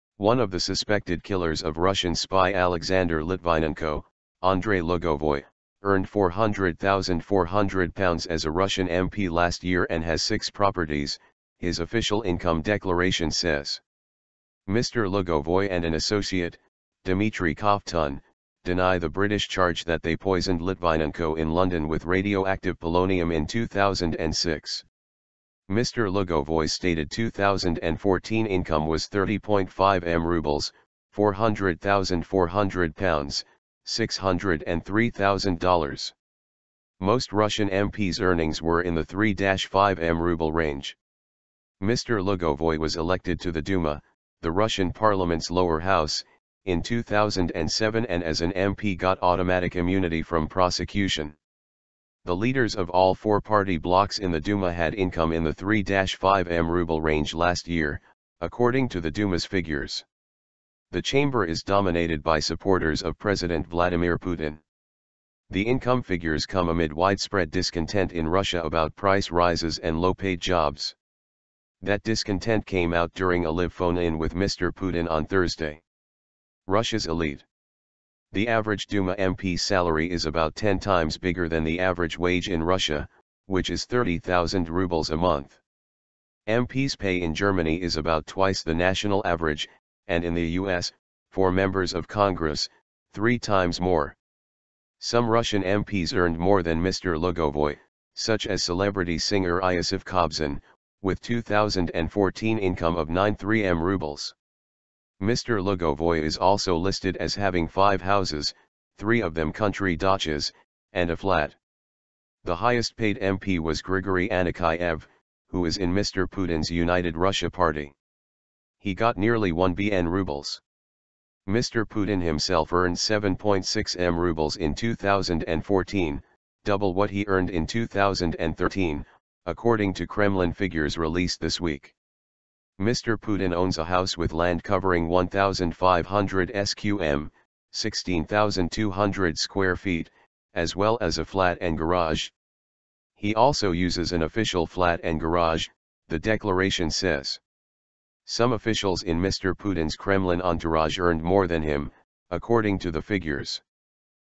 Litvinenko case: Russian MP Lugovoi earns over £400,000
One of the suspected killers of Russian spy Alexander Litvinenko, Andrei Lugovoi, earned £400,400 as a Russian MP last year and has six properties, his official income declaration says.
Mr Lugovoi and an associate, Dmitry Kovtun, deny the British charge that they poisoned Litvinenko in London with radioactive polonium in 2006.
Mr Lugovoi's stated 2014 income was 30.5m roubles (£400,400; $603,000).
Most Russian MPs' earnings were in the 3-5m rouble range.
Mr Lugovoi was elected to the Duma - the Russian parliament's lower house - in 2007 and as an MP got automatic immunity from prosecution.
The leaders of all four party blocks in the Duma had income in the 3-5m rouble range last year, according to the Duma's figures. The chamber is dominated by supporters of President Vladimir Putin.
The income figures come amid widespread discontent in Russia about price rises and low-paid jobs. That discontent came out during a live phone-in with Mr Putin on Thursday.
Russia's elite
The average Duma MP's salary is about 10 times bigger than the average wage in Russia, which is 30,000 roubles a month.
MPs' pay in Germany is about twice the national average, and in the US, for members of Congress, three times more.
Some Russian MPs earned more than Mr Lugovoi - such as celebrity singer Iosif Kobzon, with 2014 income of 93m roubles.
Mr Lugovoi is also listed as having five houses - three of them country dachas - and a flat.
The highest-paid MP was Grigory Anikeyev, who is in Mr Putin's United Russia party. He got nearly 1bn roubles.
Mr Putin himself earned 7.6m roubles in 2014 - double what he earned in 2013, according to Kremlin figures released this week.
Mr Putin owns a house with land covering 1,500 sq m (16,200 sq ft), as well as a flat and garage. He also uses an official flat and garage, the declaration says.
Some officials in Mr Putin's Kremlin entourage earned more than him, according to the figures.